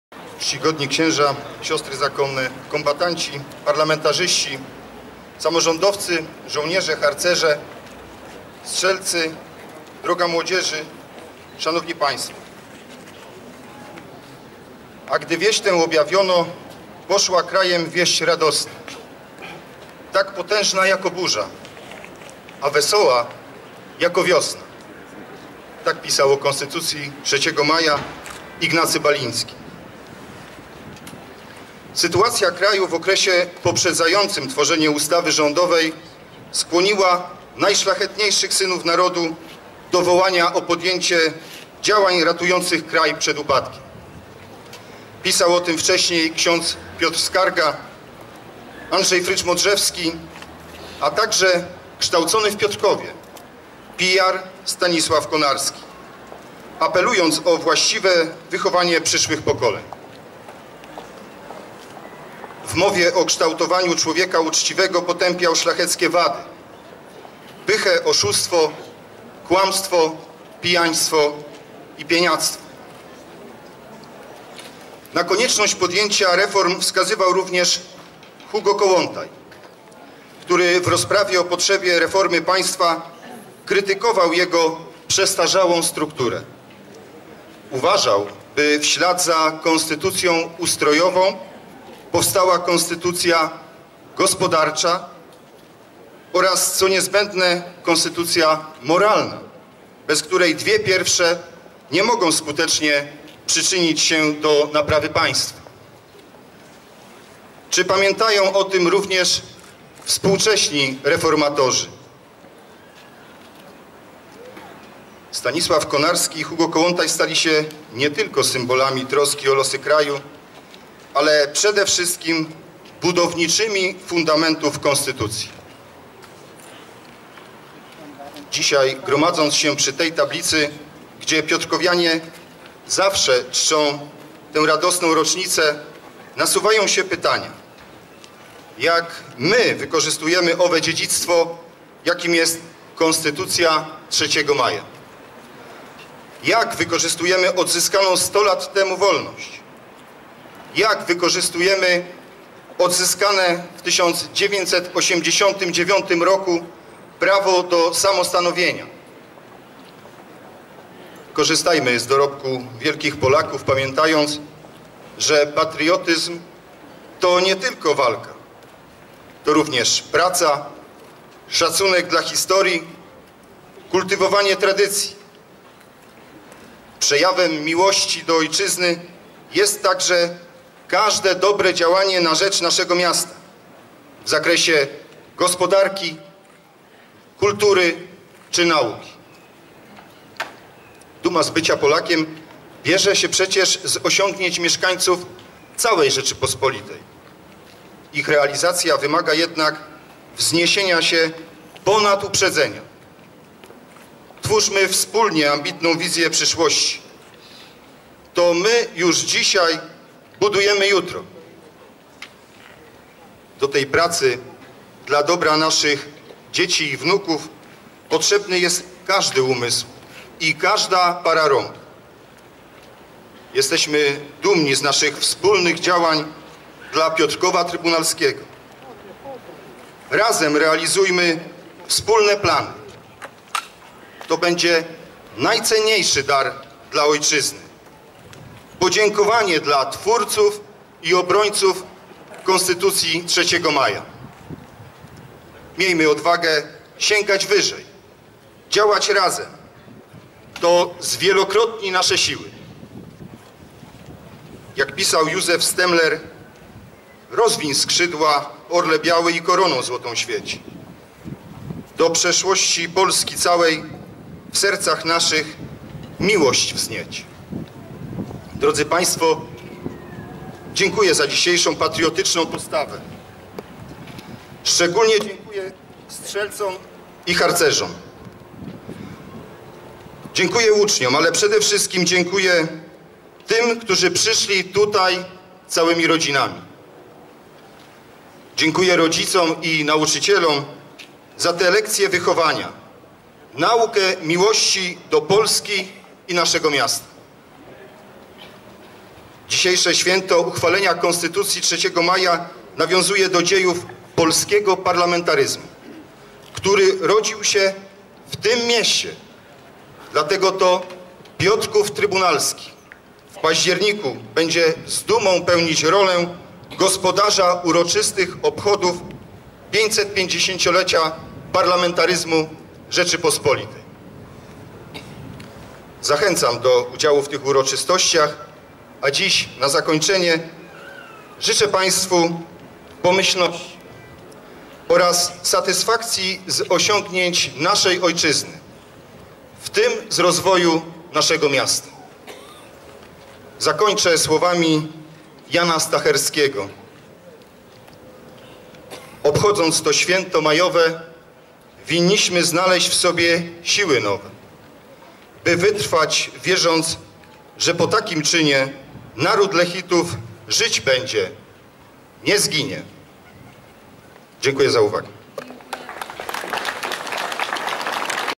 Krzysztof Chojniak Prezydent Piotrkowa 3 05 2018
Wystąpienie okolicznościowe Krzysztofa Chojniaka, Prezydenta Piotrkowa Trybunalskiego z okazji 227. rocznicy uchwalenia Konstytucji 3 Maja